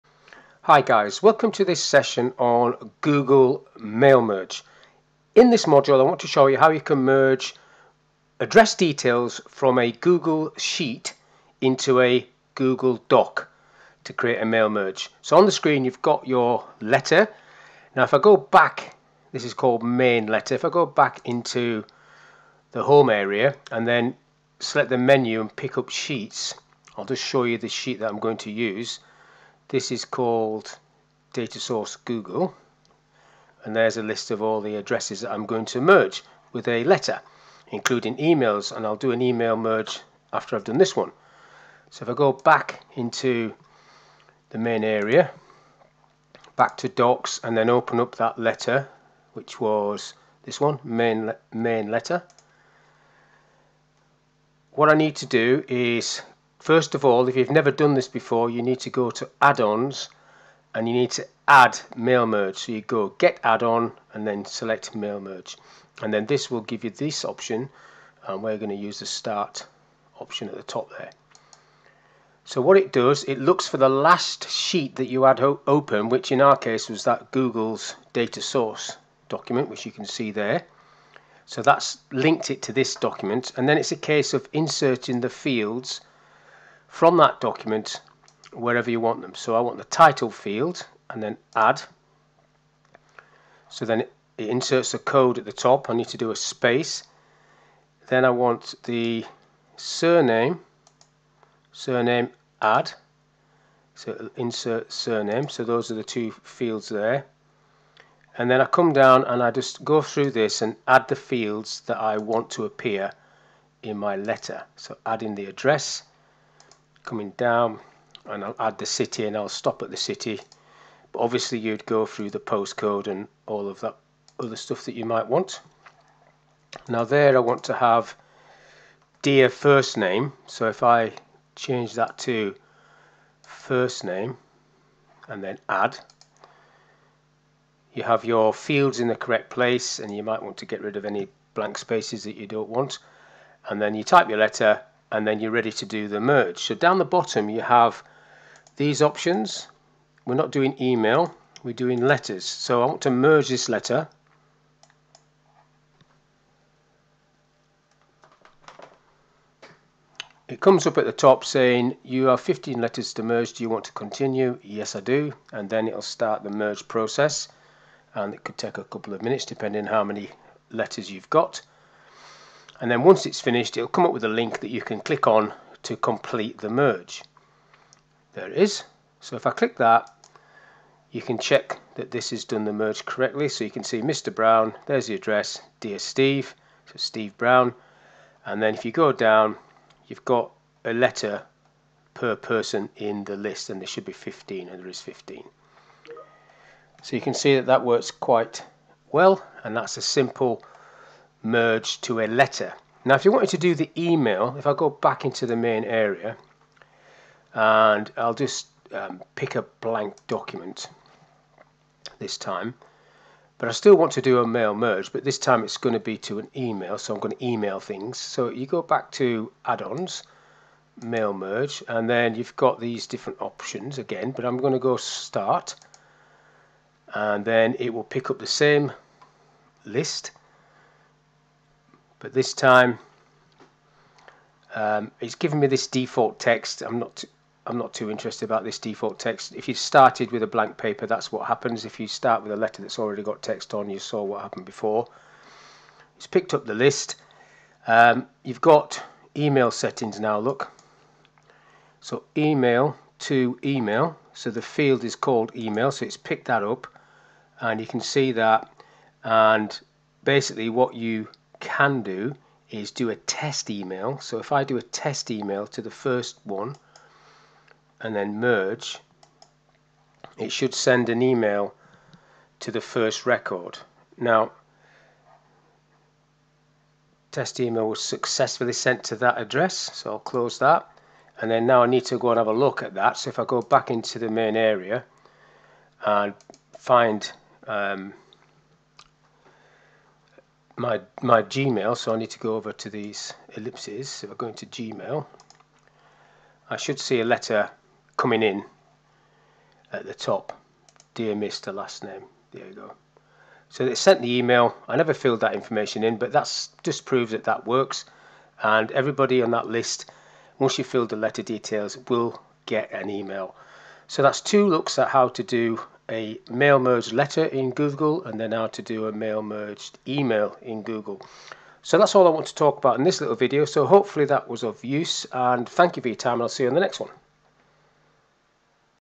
Mail merge in Google. Google sheets to Google Docs. Mail Merge
This video explains how to mail merge in Google. Google sheets to Google Docs. The tutor explains how to get the mail merge add on in Google docs and then goes through the process of the mail merge. the first step is to open the data source in Google sheets that you want to merge. The second step is to start the mail merge process in Google Docs. Once the mail merge process starts Google Docs will pick up the last Sheet that was opened and use it as the data source. The tutor then explains how to add all the fields into the document. Once all the fields from the data source are added the merge can be completed and checked. The final part of the video covers how to mail merge an email message and check that it works. This is a Google Docs and Sheets tutorial covering mail merge, merge to a letter, merge to email and data source.